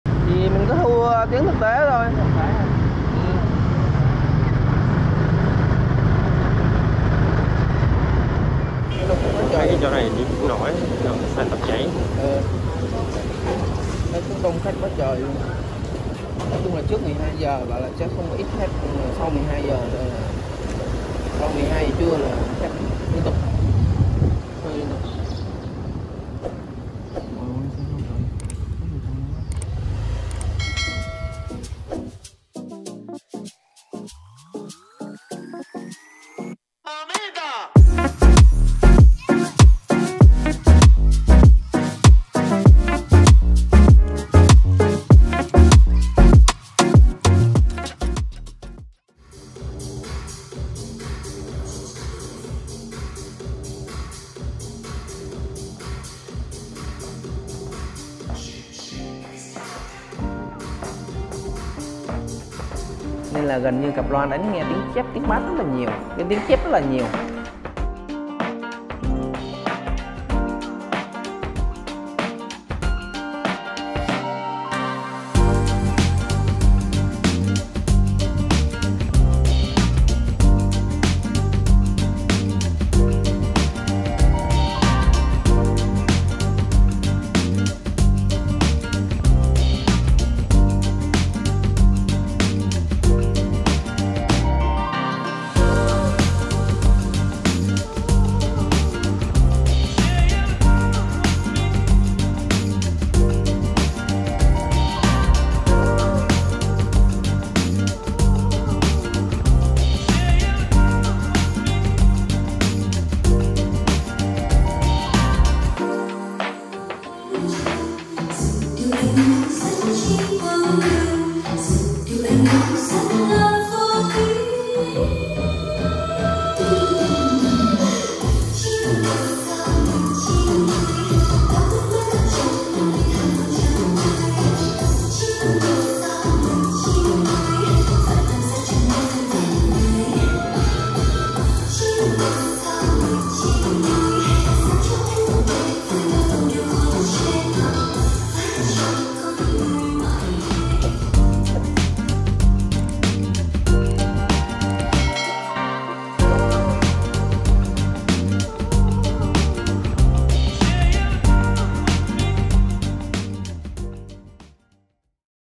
Lắp Đặt Hệ Thống Âm Thanh Loa Column Array 100 Triệu Cho Phòng Tập Nhảy UC Dance Space | Tập 2
Lắp Đặt Hệ Thống Âm Thanh Loa Column Array 100 Triệu Cho Phòng Tập Nhảy UC Dance Space